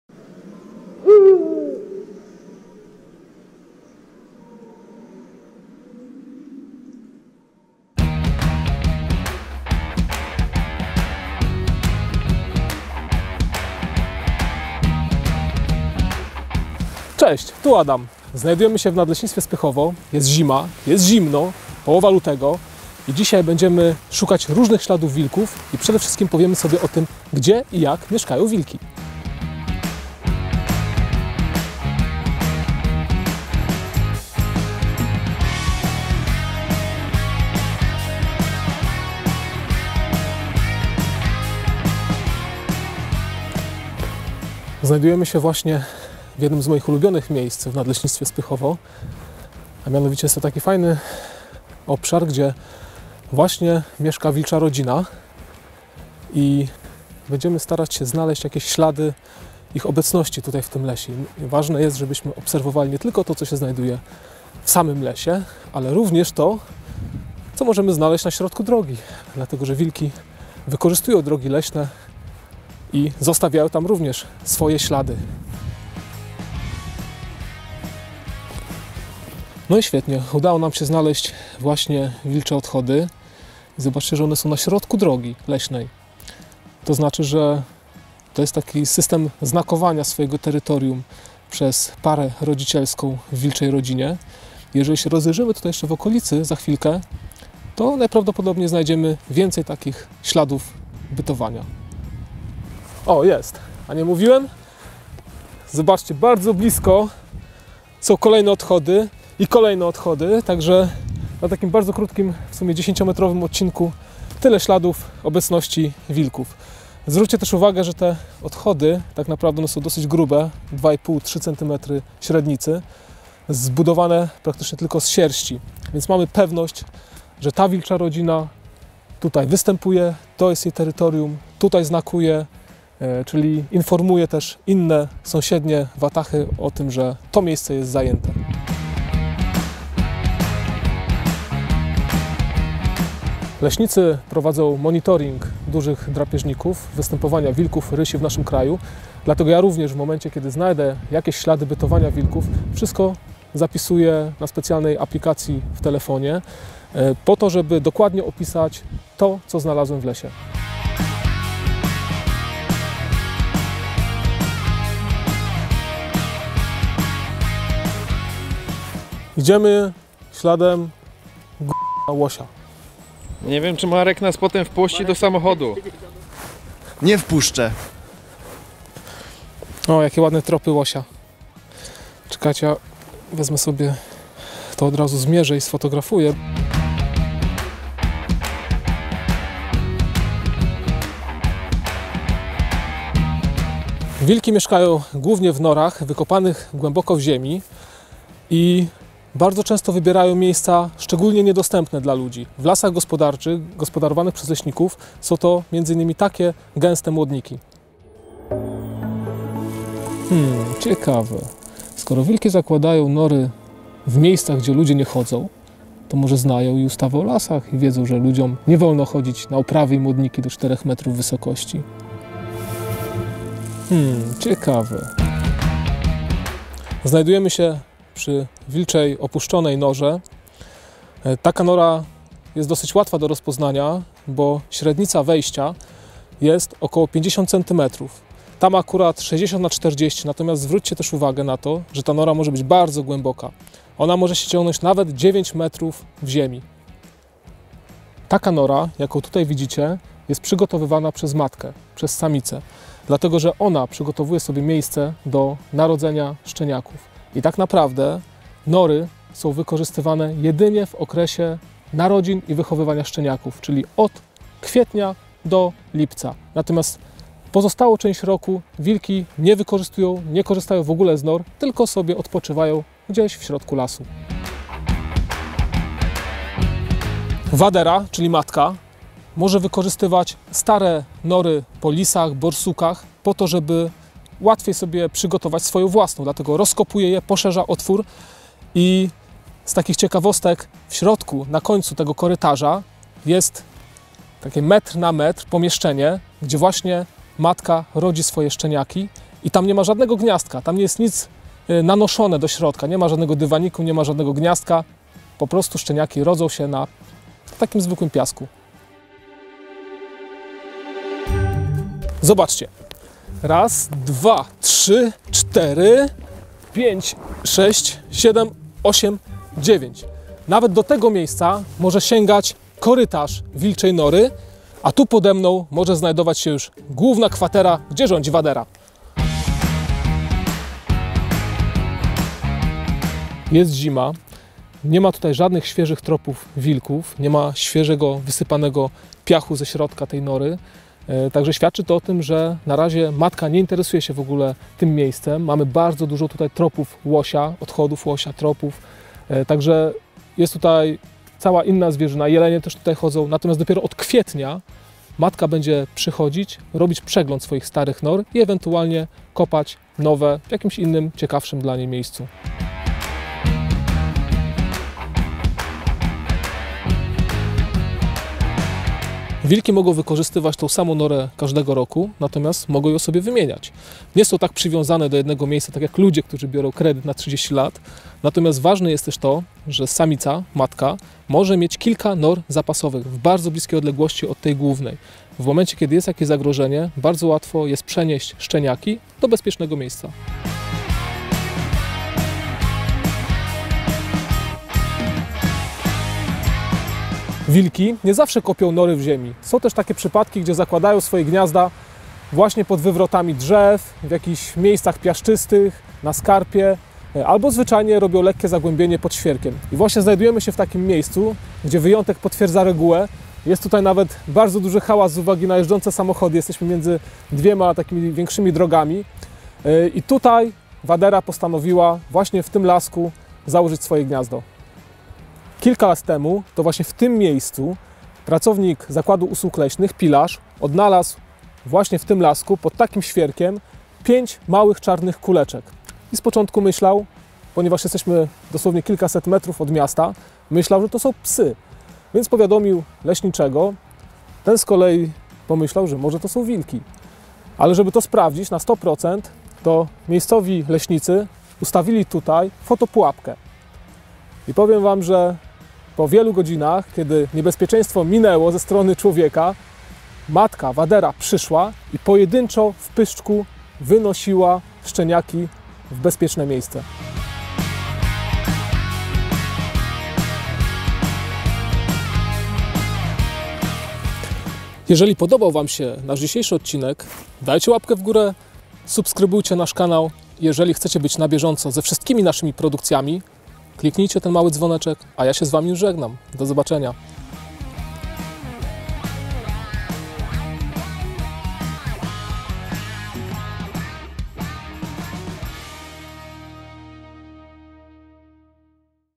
Gdzie i jak mieszkają wilki? | Oblicza lasów #74
Jakie ślady pozostawiają po sobie wilki? W jaki sposób samice wilków przygotowują sobie miejsce do narodzin szczeniaków? Czy wilki wykorzystują nory po innych zwierzętach? O tym wszystkim i nie tylko opowiada w dzisiejszym odcinku Adam z Nadleśnictwa Spychowo.

Prowadzący: Adam Gełdon
Reżyseria: Tomasz Zielonka
Zdjęcia: Adrian Frankowski
Zdjęcia lotnicze: Marek Matecki